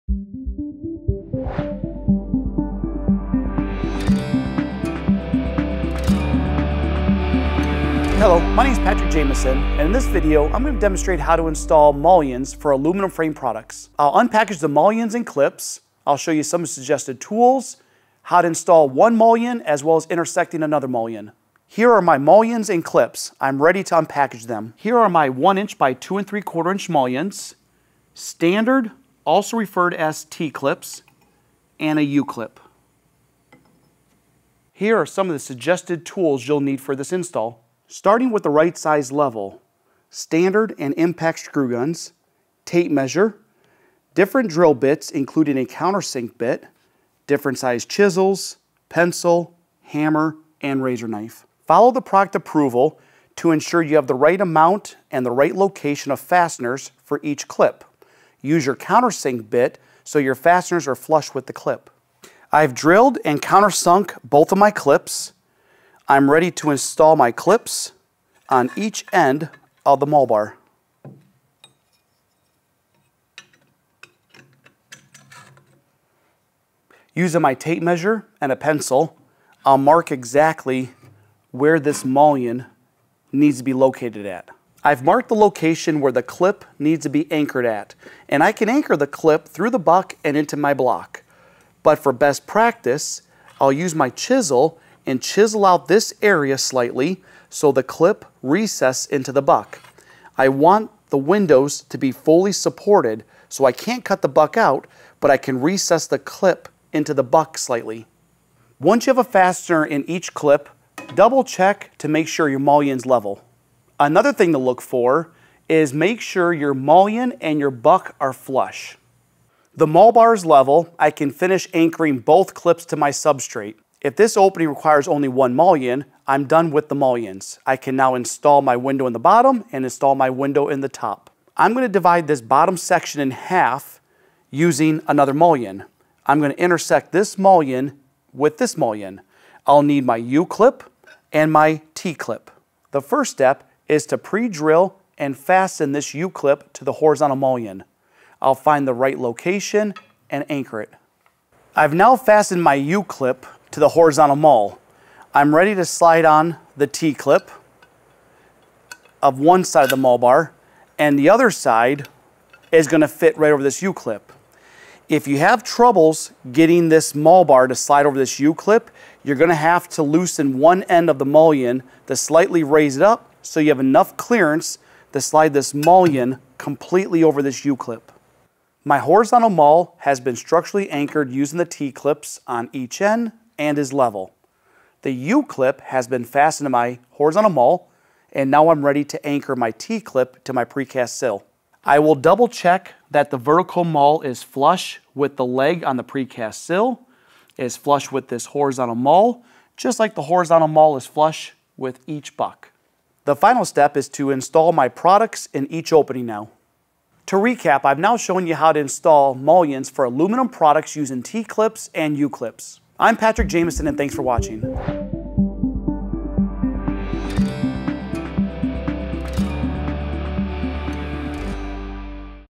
PGT Aluminum Mullion Installation
This quick video shows the basics to install aluminum mullions (mull bars).